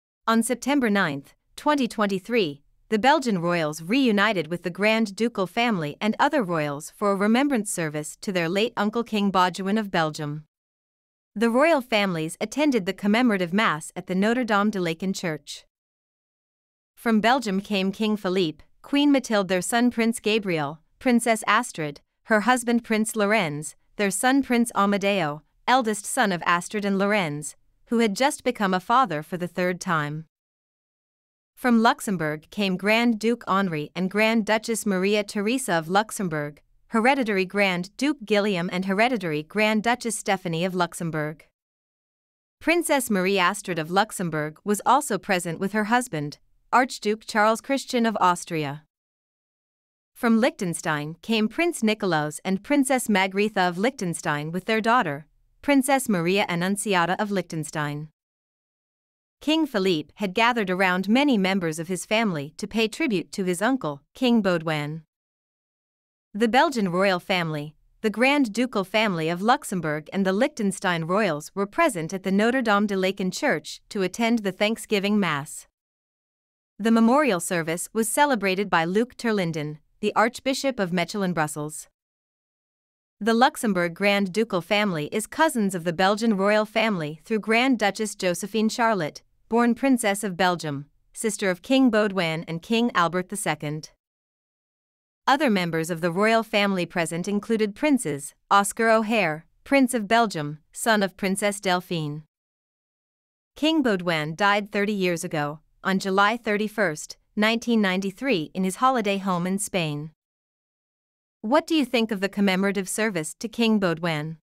Foreign royals join belgian royals at the thanks giving service for king Baudouin
On 9th september 2023, the belgian royals reunited with the grand ducal family and other royals for a remembrance service to their late uncle king bauduoin of belgium at the Notre-Dame de Laeken church. #